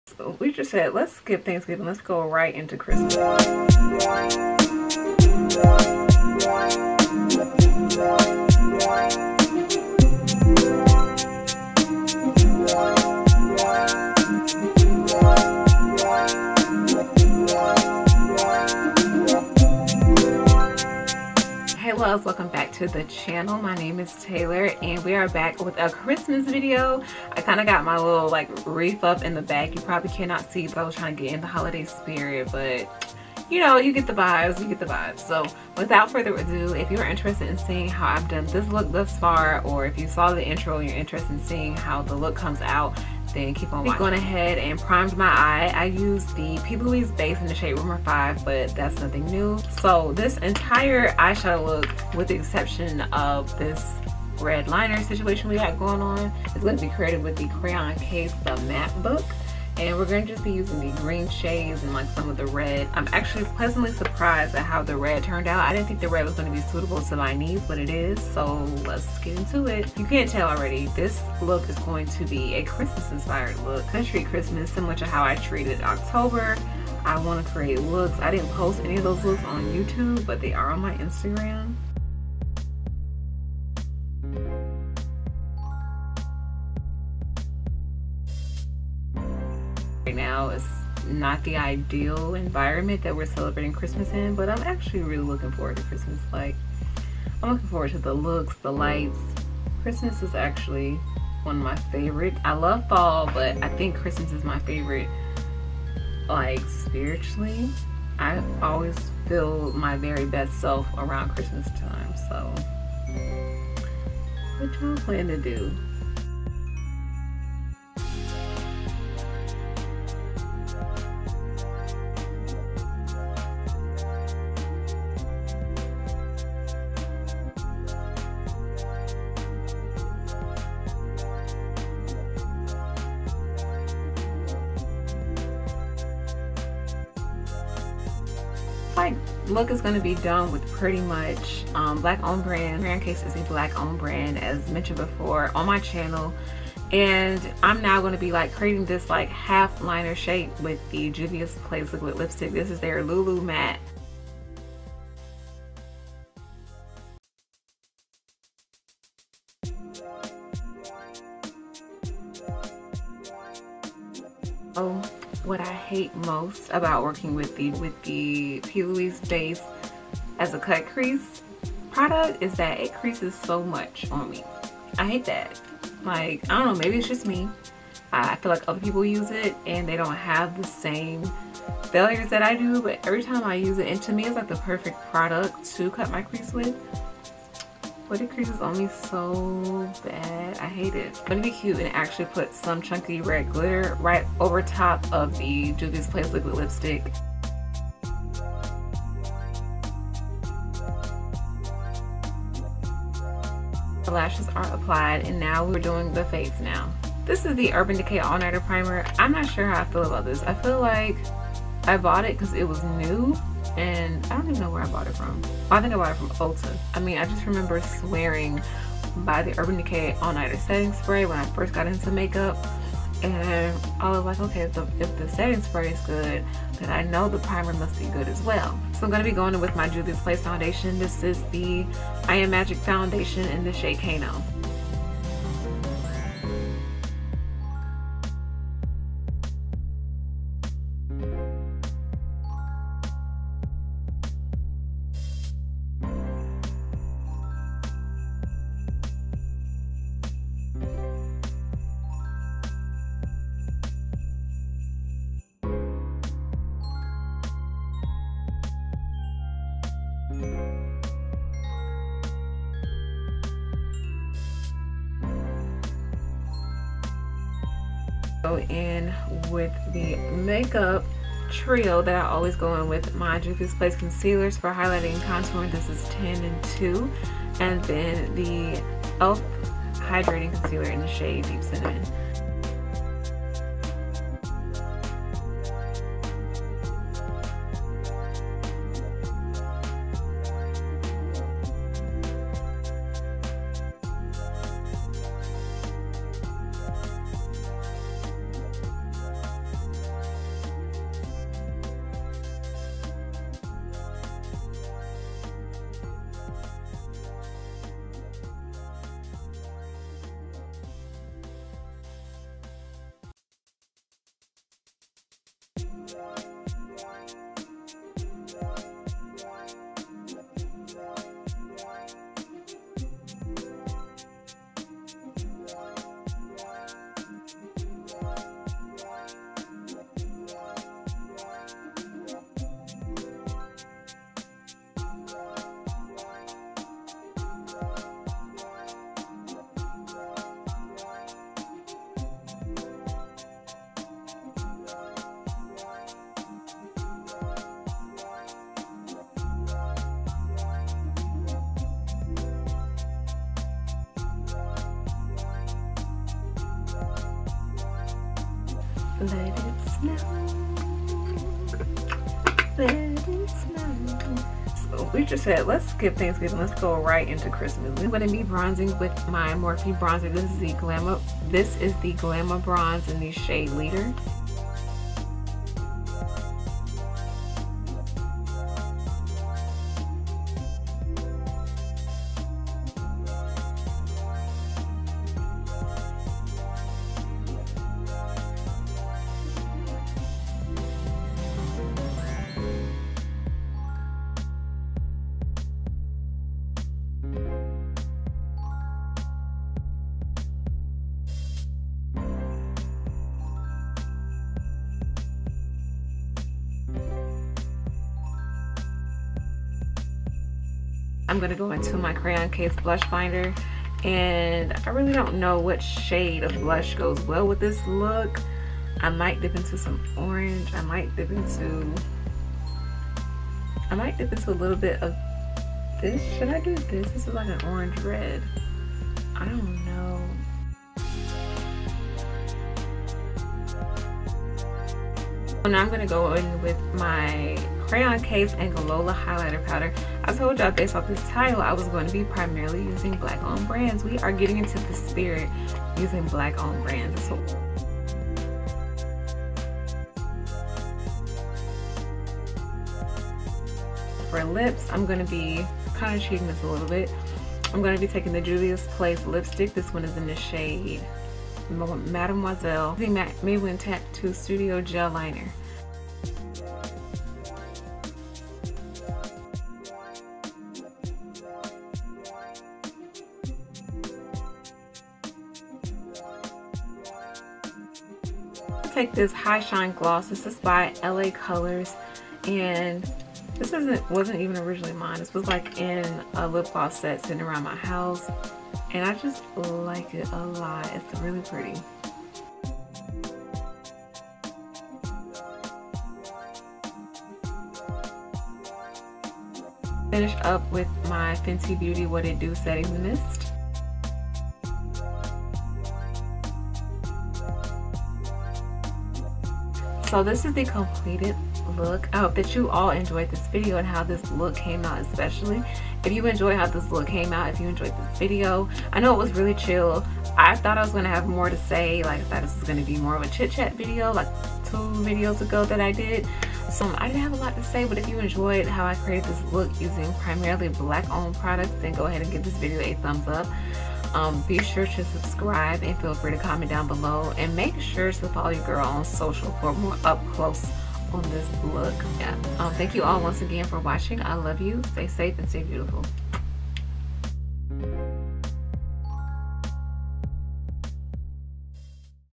BLACK-OWNED BRANDS YOU NEED FOR THE HOLIDAY | TRANSFORM WITH ME 🎄
Hey loves 💕🎄 welcome to my first official holiday video! Not sure if I’m participating in vlogmas, but I wanted to put out Holiday content. Enjoy ❤️❤️